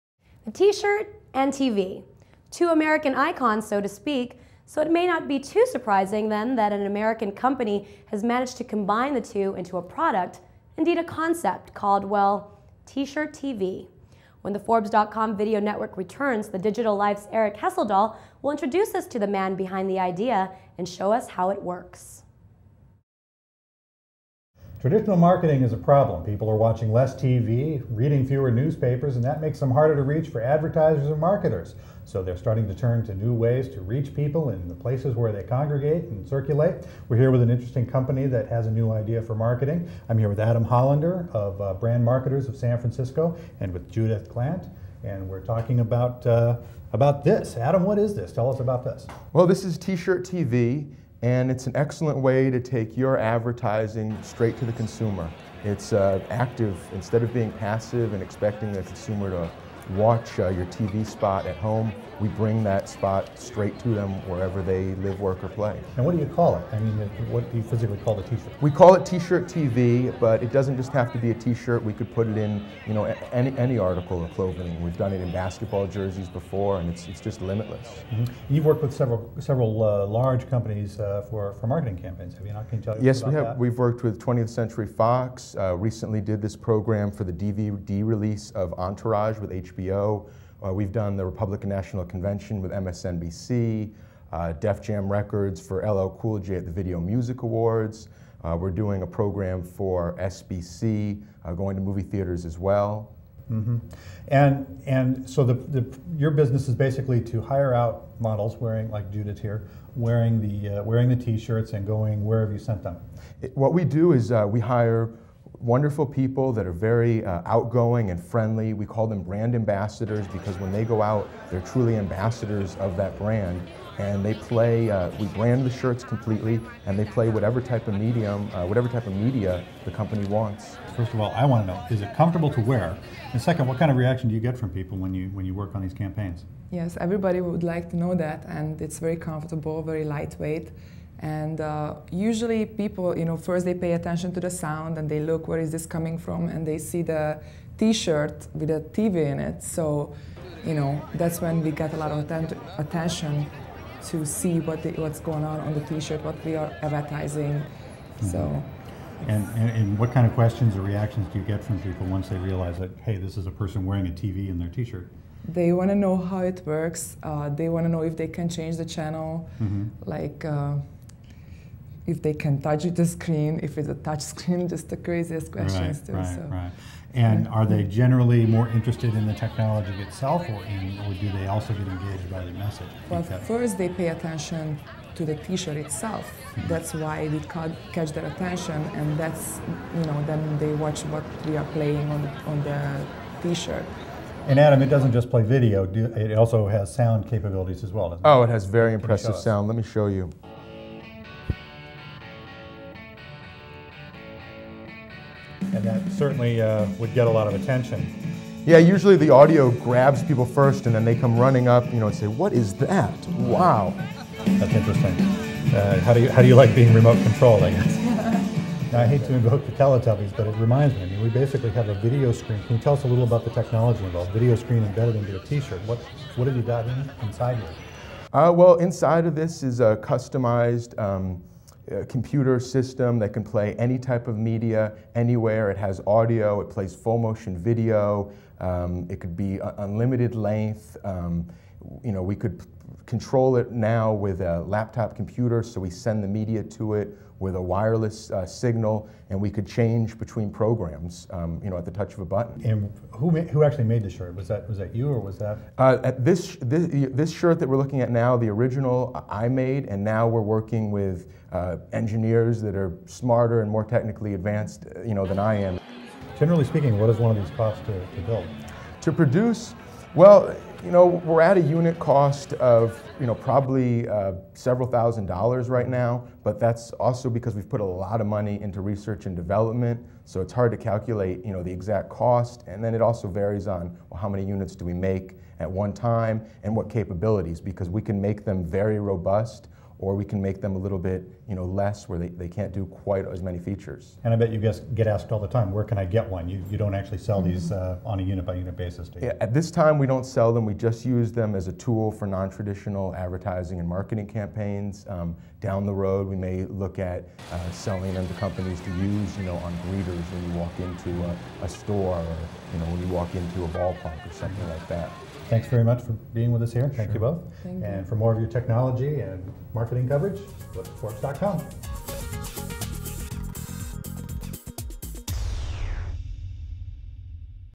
T Shirt TV® Interview Forbes
Forbes interviews Adam Hollander about T-Shirt TV®